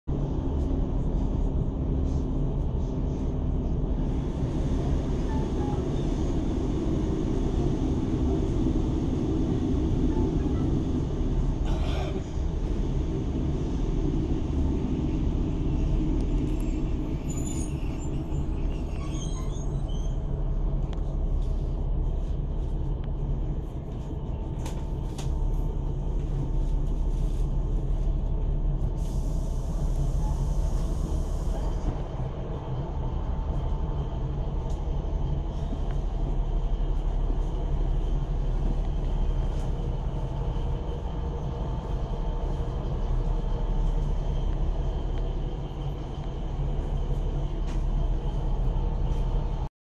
Camborne Train Station
Train Service From Penzance To Plymouth Calling At Camborne